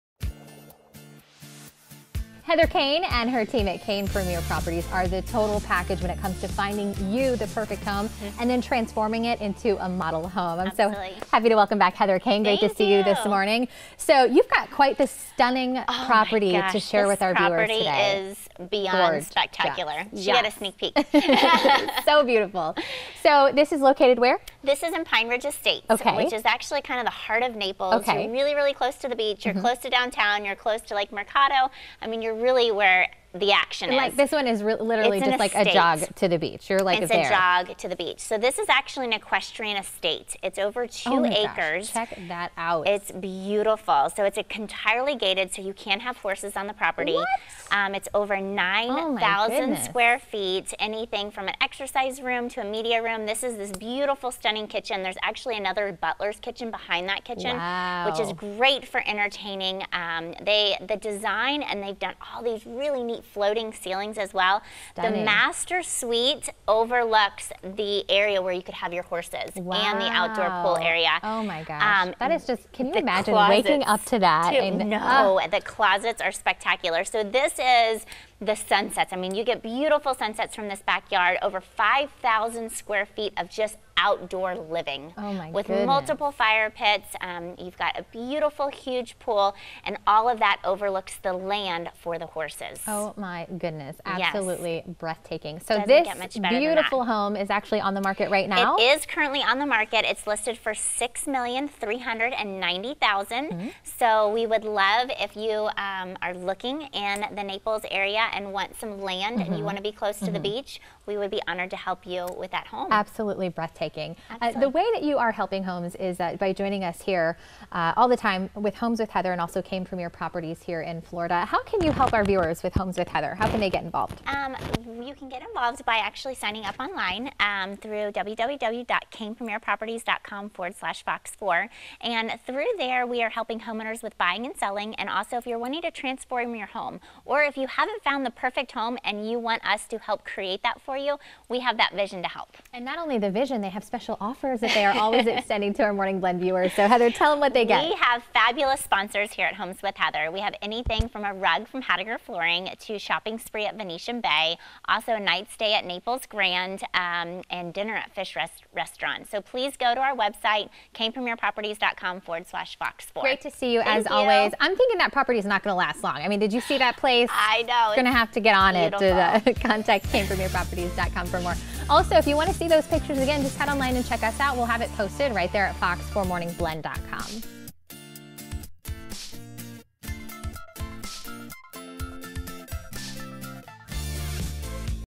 Homes With Heather Caine!
We look at some more homes with Heather and how she can help you create your dream home!